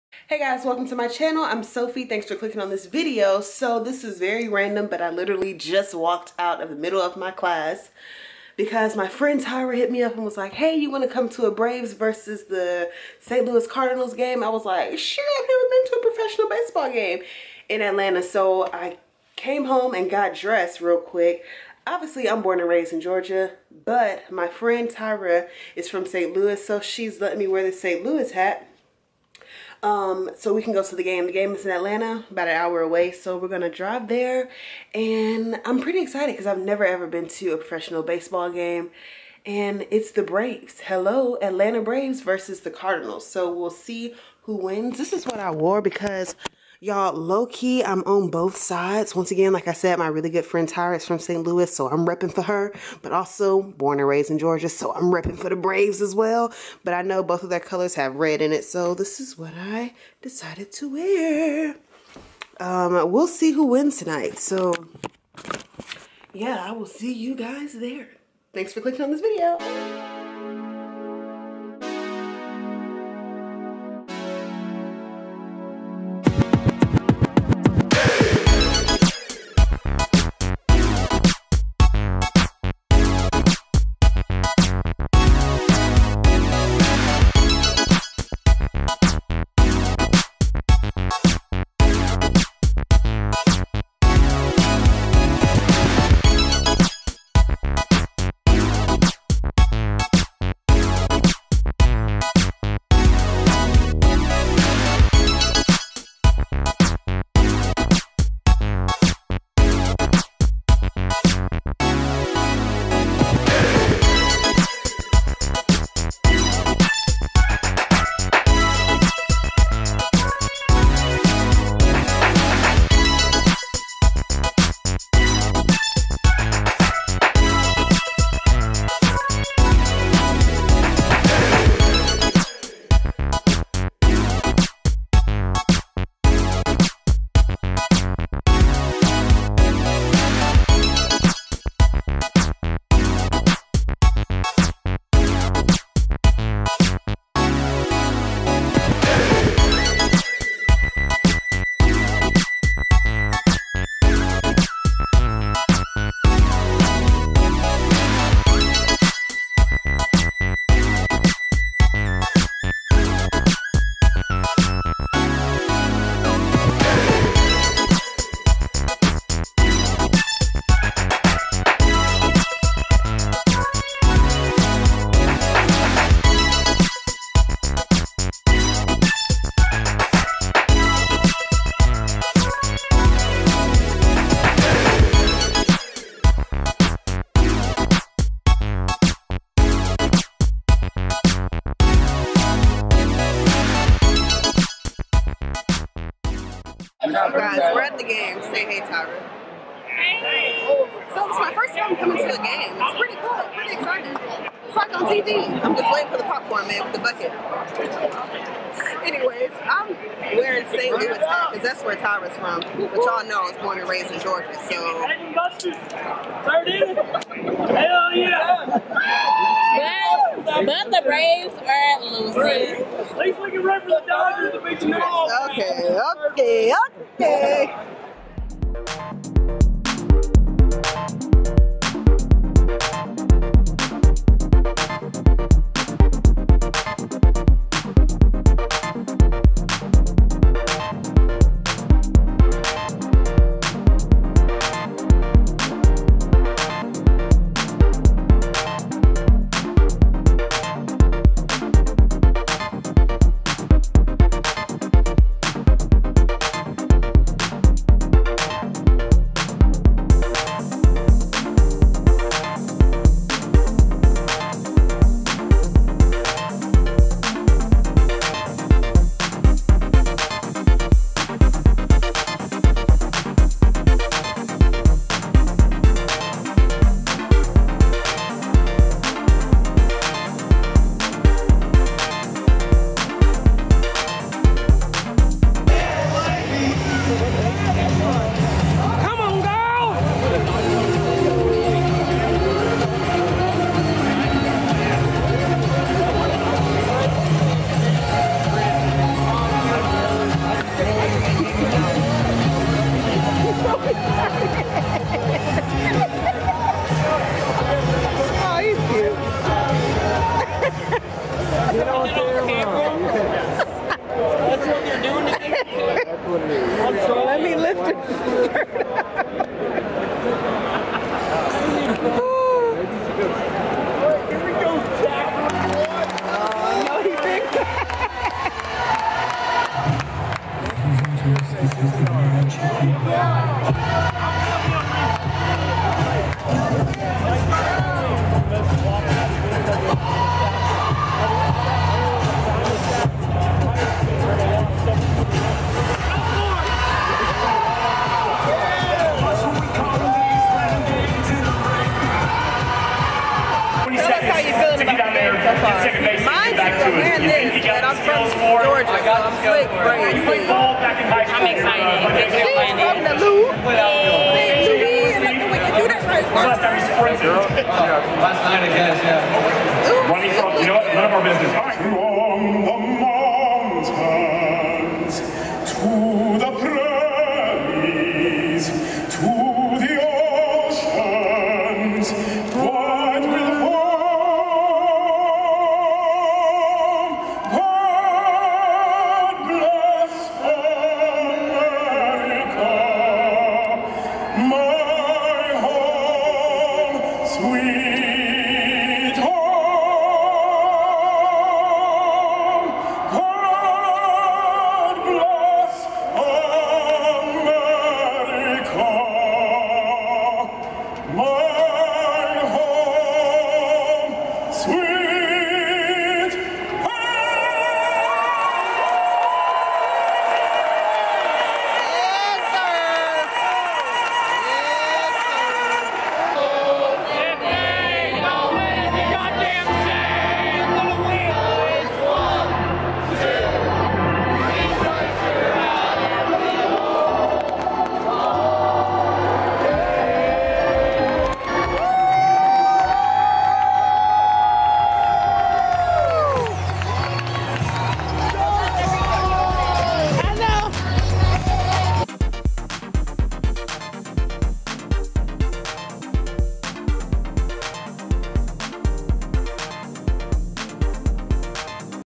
VLOG: 1ST BASEBALL GAME EVER! BRAVES vs. CARDINALS
HEY THX FOR WATCHING THIS VID! I really appreciate it.

- HELP ME REACH 1K SUBBIES BY 2020!! -

 LOVE.  PEACE.  &  POSITIVITY.